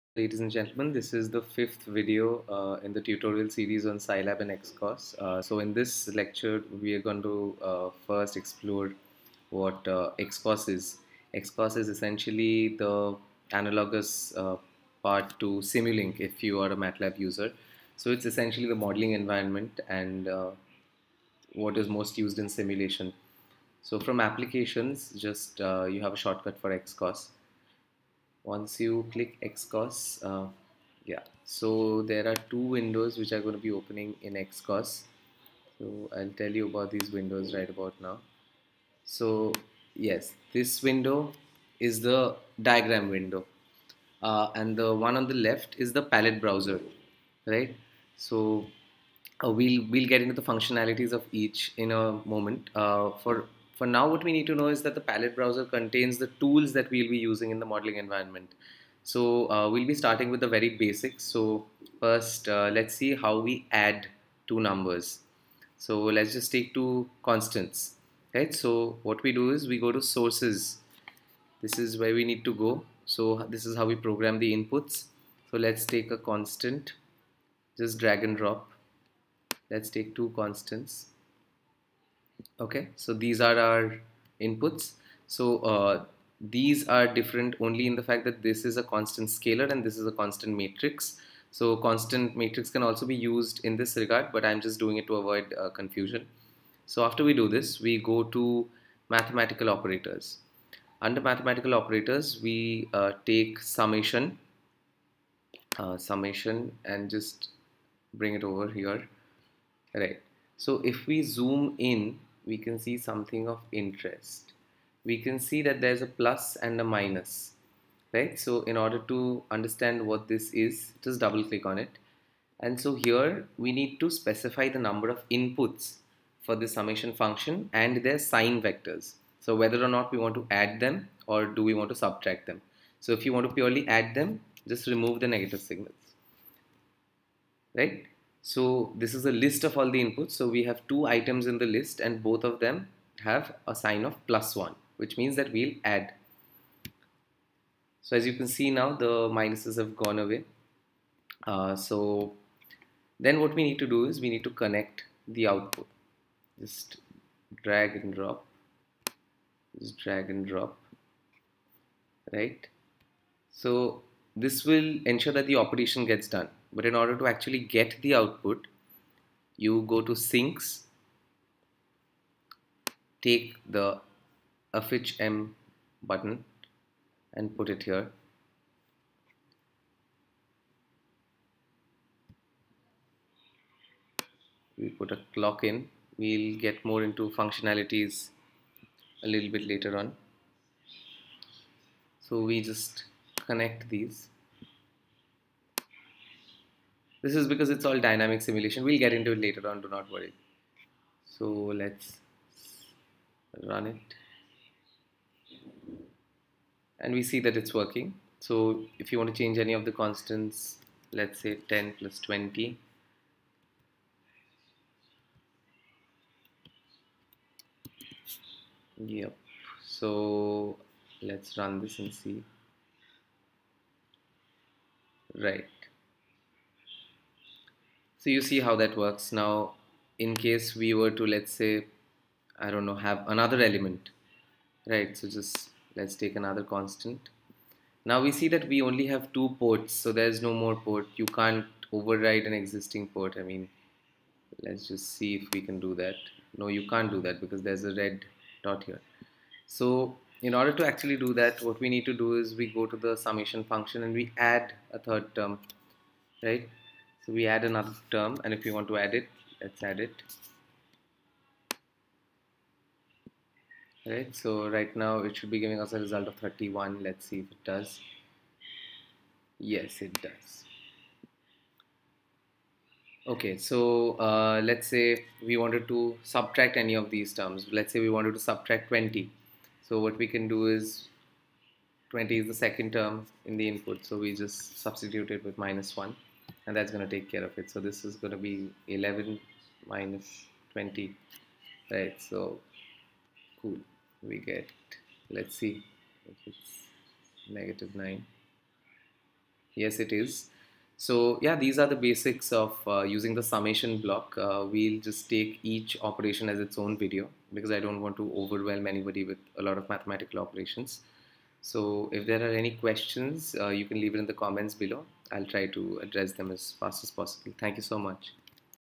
5. Introduction to Xcos Modelling Environment | Souryadeep Basak
We sneak a peek into the world of Xcos and get started with basic mathematical operations.